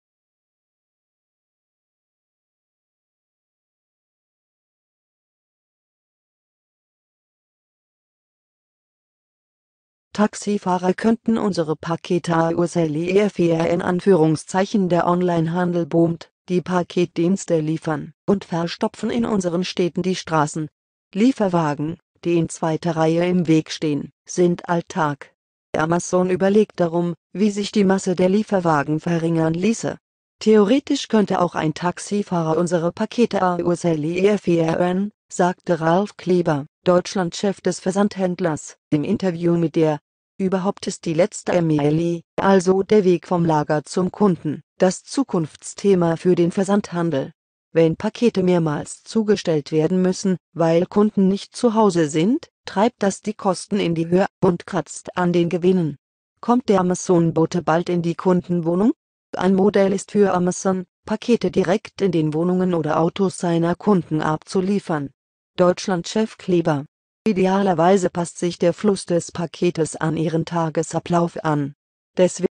„Taxifahrer könnten unsere Pakete ausliefern“
Der Online-Handel boomt, die Paketdienste liefern – und verstopfen in unseren Städten die Straßen. Lieferwagen, die in zweiter Reihe im Weg stehen, sind Alltag.

Amazon überlegt darum, wie sich die Masse der Lieferwagen verringern ließe. „Theoretisch könnte auch ein Taxifahrer unsere Pakete ausliefern“, sagte Ralf Kleber, Deutschland-Chef des Versandhändlers, im Interview mit der.

Überhaupt ist die „letzte Meile“, also der Weg vom Lager zum Kunden, DAS Zukunftsthema für den Versandhandel. Wenn Pakete mehrmals zugestellt werden müssen, weil Kunden nicht zu Hause sind, treibt das die Kosten in die Höhe und kratzt an den Gewinnen.

Kommt der Amazon-Bote bald IN die Kunden-Wohnung?

Ein Modell ist für Amazon, Pakete direkt in den Wohnungen oder Autos seiner Kunden abzuliefern. Deutschland-Chef Kleber: „Idealerweise passt sich der Fluss des Paketes an Ihren Tagesablauf an. Deswegen testen wir in den USA beispielsweise die Zustellung in den Kofferraum oder hinter die Haustür.“

Technisch sei das „kein Problem“ und die Amerikaner seien „vielleicht auch etwas experimentierfreudiger“, sagt Kleber. „Wobei wir herausgefunden haben, dass Sicherheitsbedenken bei den Deutschen gar nicht so sehr im Vordergrund stehen. Die schämen sich eher, wenn nicht aufgeräumt ist.“